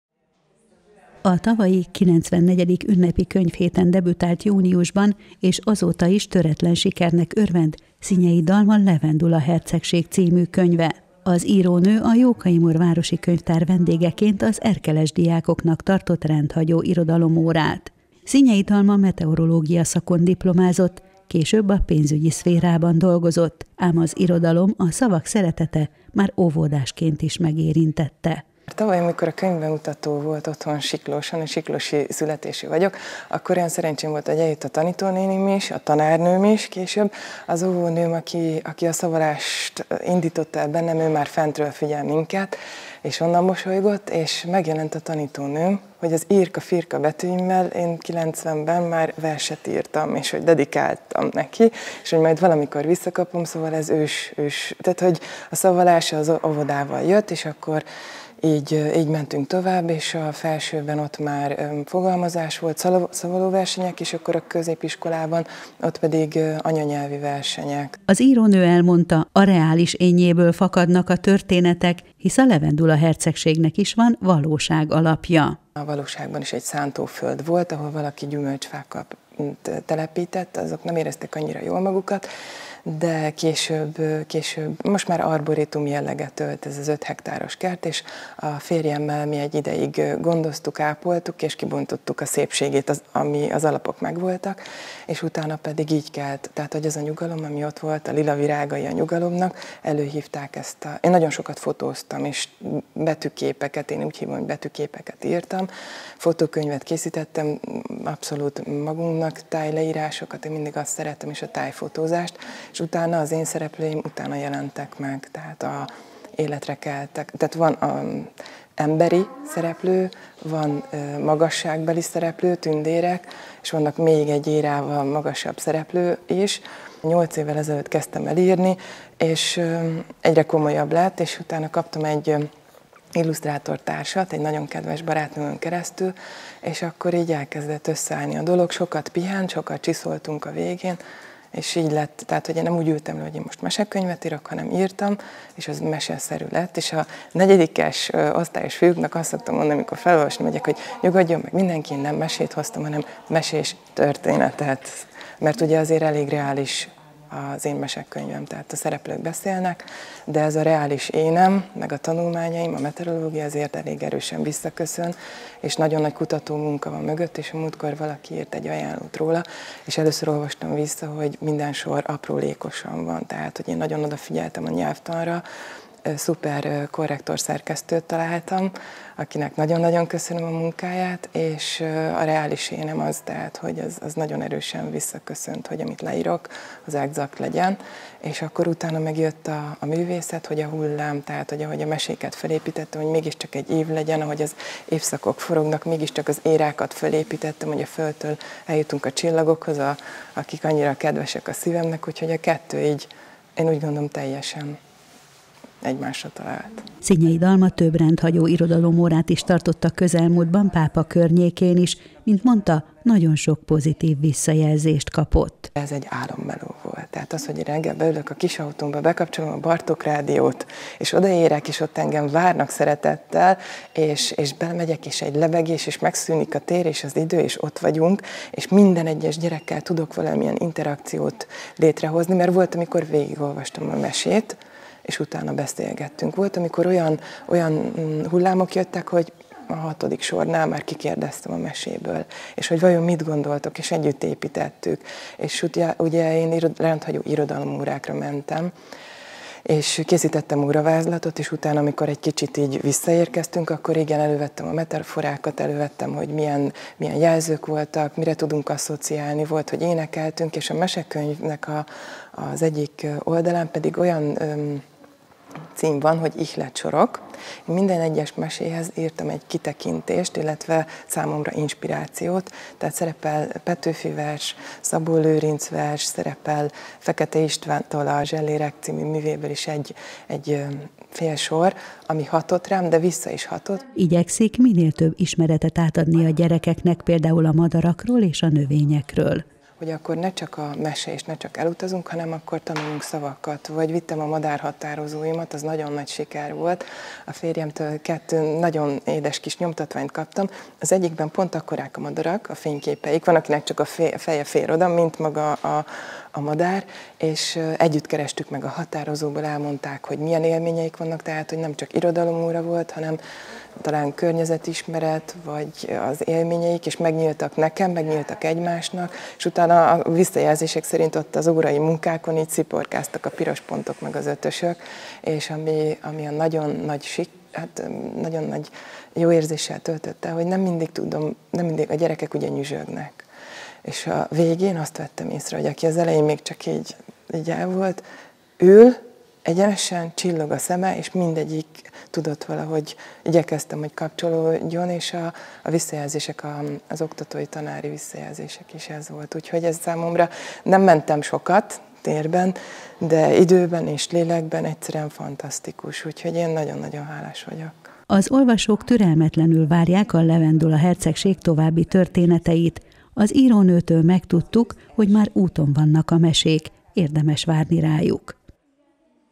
Szinyei Dalma Levendula Hercegsége
A tavalyi, 94. Ünnepi Könyvhéten debütált júniusban és azóta is, töretlen sikereknek örvend Szinyei Dalma Levendula Hercegség című könyve. A történet valóságon alapul, minden az írónő eddig megszerzett tudásából, tapasztalatából vagy akár egy-egy gyerekkori emlékéből fakad, s mindent áthat a béke és a lila levendulák érezhető illata. Szinyei Dalma a Jókai Mór Városi Könyvtár vendégeként az erkeles diákoknak tartott rendhagyó irodalomórát.